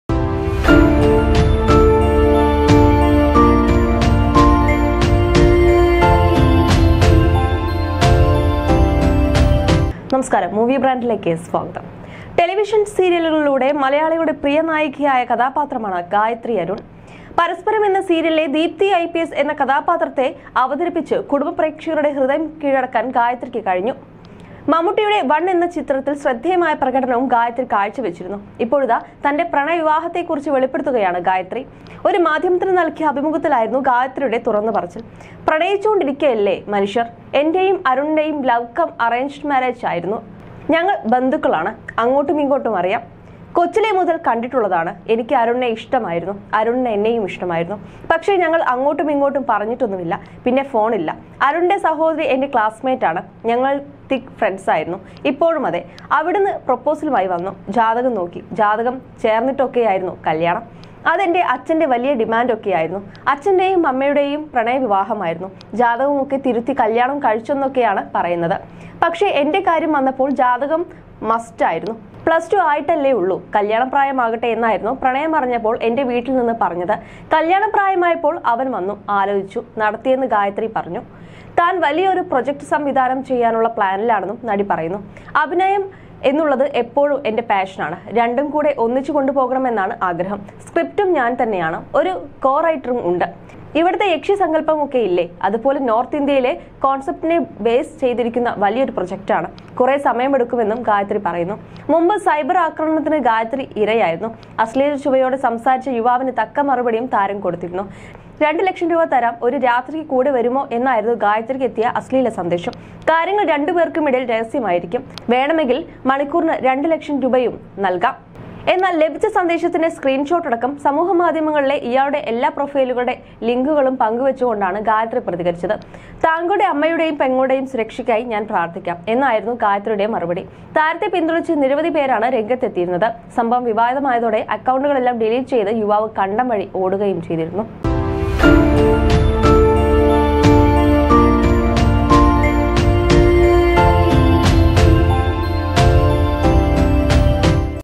GayathriArun : അതിനപ്പുറം ഉള്ള ചോദ്യങ്ങൾ വന്നാൽ പ്രതികരിക്കും...
ജാതകമൊക്കെ തിരുത്തി കല്യാണം കഴിച്ചെന്നൊക്കെയാണ് പറയുന്നത്. പക്ഷേ എന്റെ കാര്യം വന്നപ്പോൾ ജാതകം മസ്റ്റായിരുന്നു...